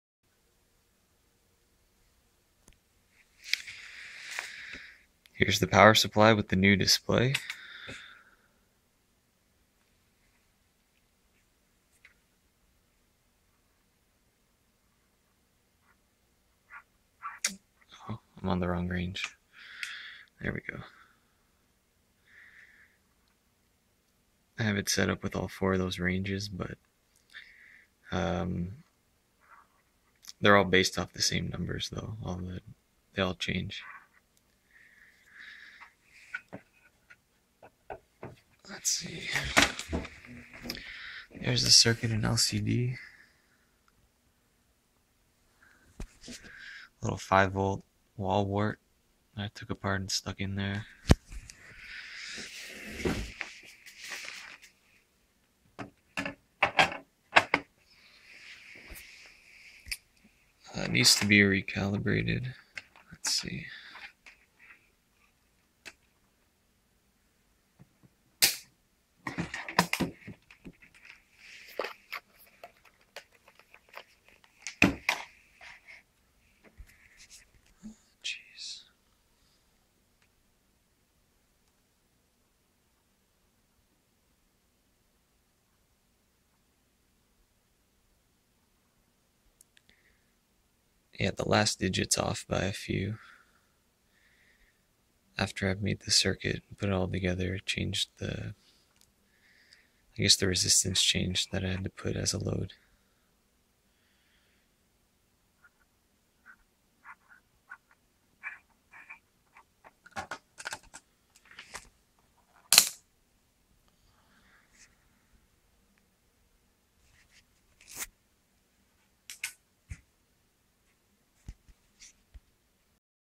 Power Supply Meter Replacement - MSP430 and 16x2 LCD
I have an old hp/agilent power supply from the 60s that still proves to be useful. The only problem was that the VU meter was broken. With all my spare time I decided to turn it into a project for me.

Due to its ancient (but still good) analog circuitry, it wasn't easy. First, there was nowhere for me to get a constant DC power source for the mcu and lcd, and second, the voltages generated are all negative relative to ground.

I solved these using a small 5V ac adapter that I stuck by the power switch, and using a chip with an sd16 adc (msp430f2013). The sd16 allowed me to measure the voltage drop across a resistor on the two meter leads without having to worry about having a common ground with the mcu.

Anyways, I will post the code eventually, but it's nothing too special.